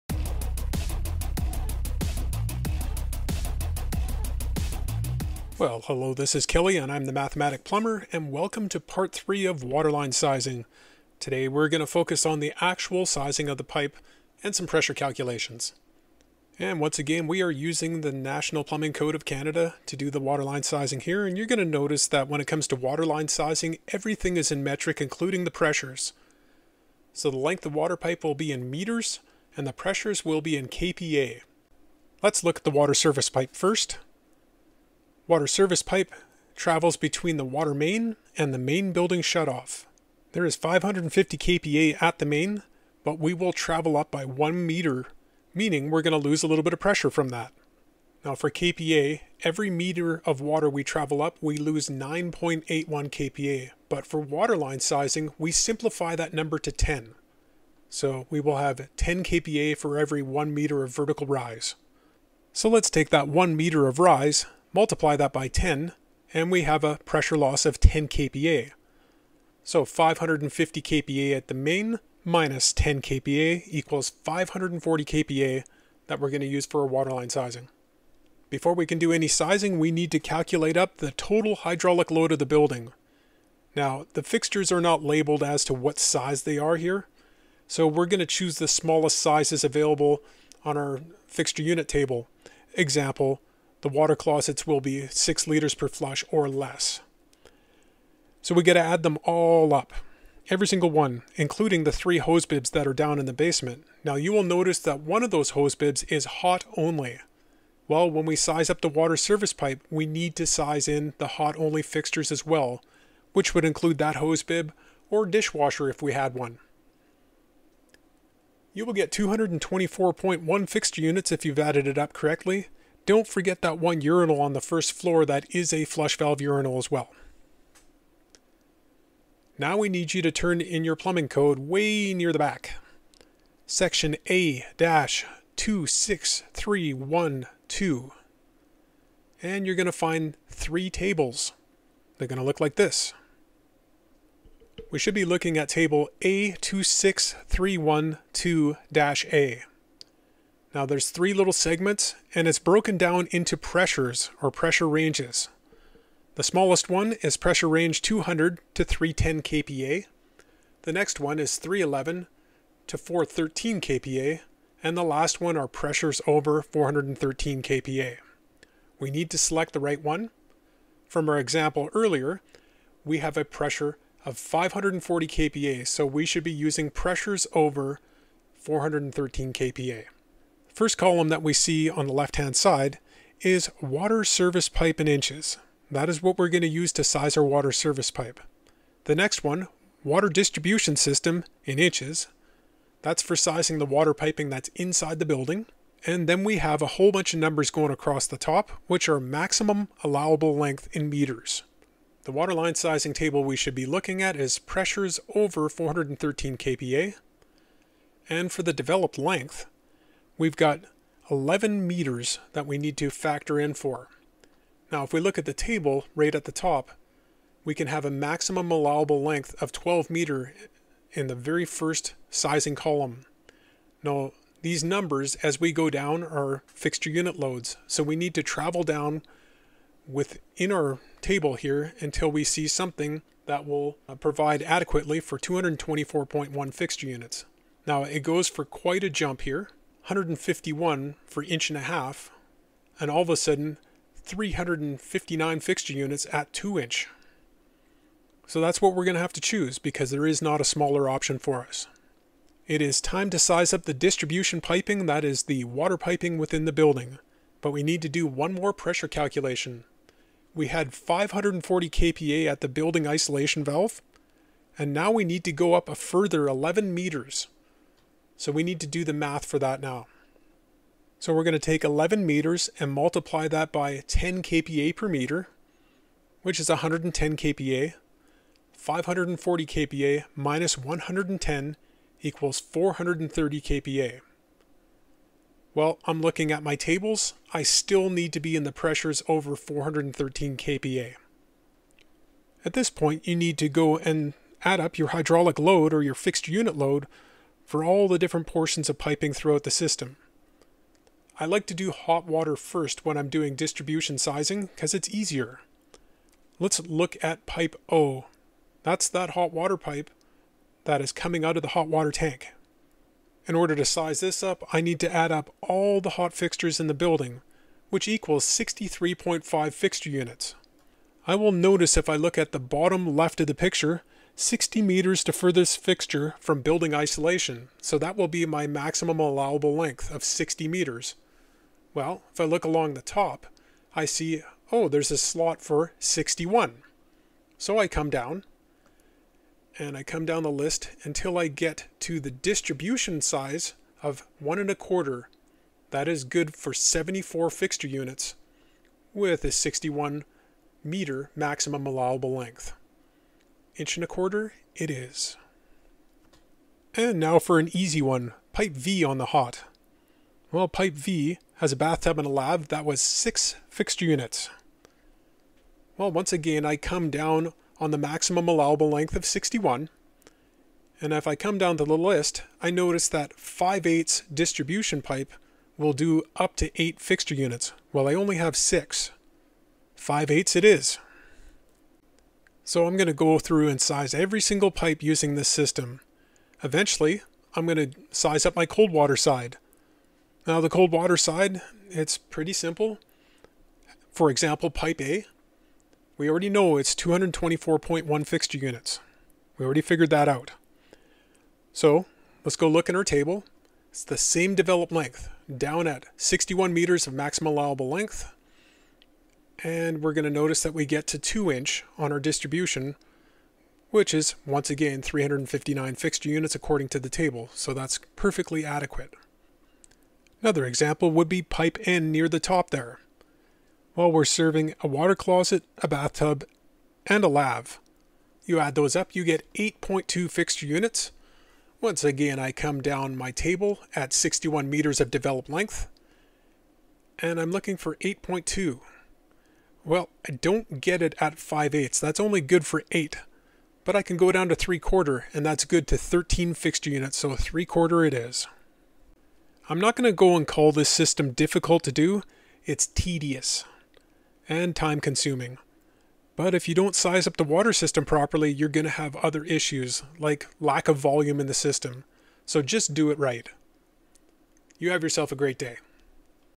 Water Line Sizing Part: 3
This video describes how to size water lines in a small commercial application.